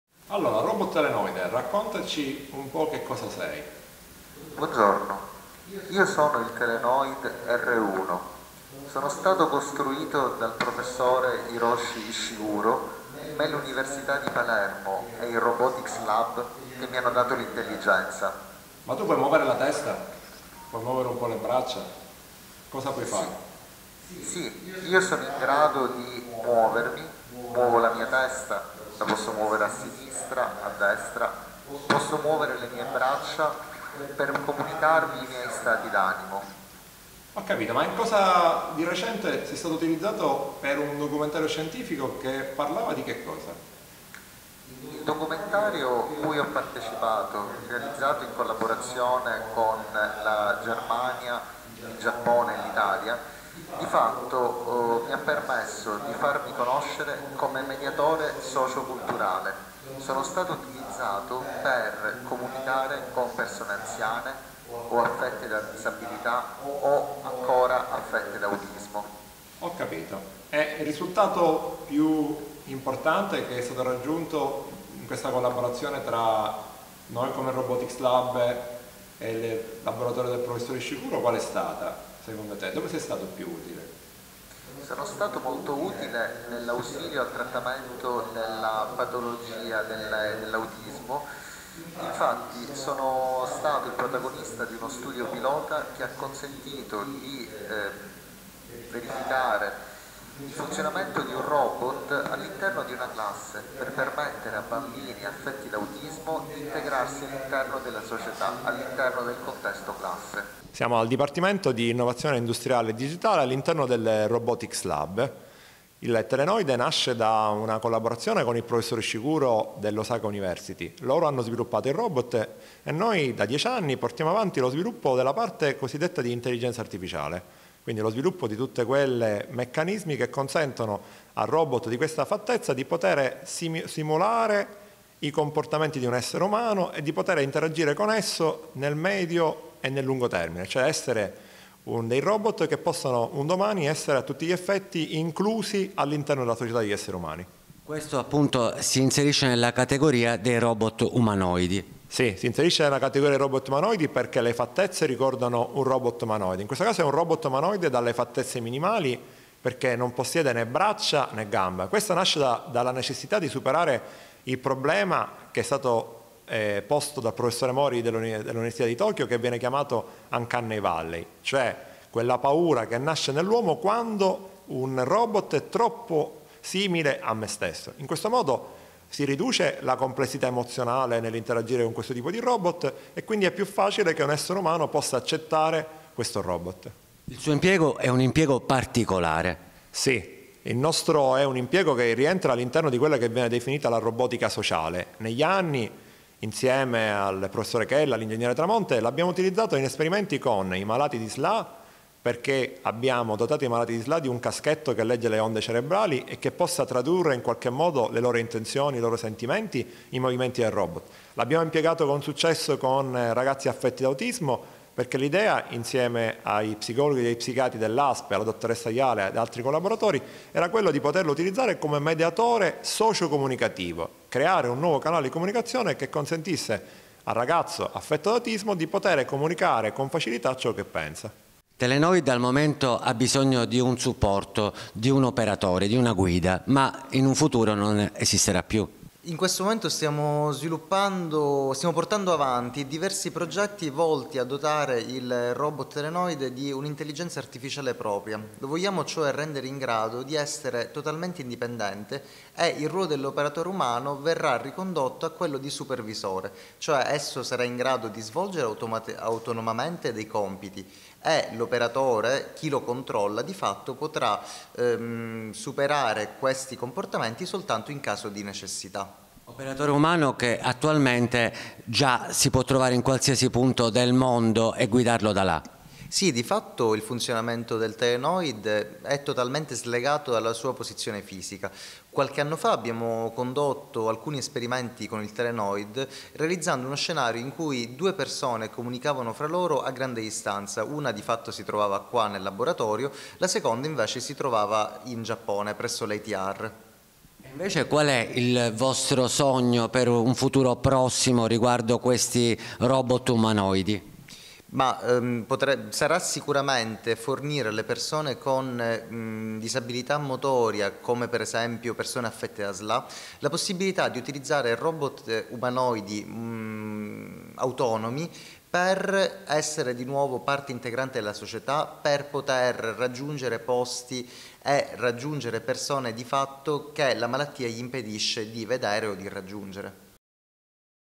Telenoid un geniale robot umanoide, creato in Giappone ma dall’intelligenza tutta siciliana.
E’ nel dipartimento Innovazione Industriale e Digitale dell’Università degli Studi di Palermo che i ricercatori di Roboticslab forniscono intelligenza artificiale al robot creato in Giappone; una collaborazione che ha permesso di creare una macchina in grado di supportare persone affette da Sla e bambini con autismo attraverso l’intervento di un operatore esterno che di fatto interagisce con la macchina.
Su questa particolare macchina artificiale la tv Giapponese ha realizzato un documentario scientifico.
Lui è in grado di riprodurre fedelmente voce, movimenti della testa e la velocità di parola dell’operatore che a distanza anche di migliaia di chilometri impartisce i comandi.
Uno strumento che apre nuove frontiere alla comunicazione e alla socializzazione dei bimbi con autismo e che nel futuro prossimo potrà essere reso totalmente autonomo.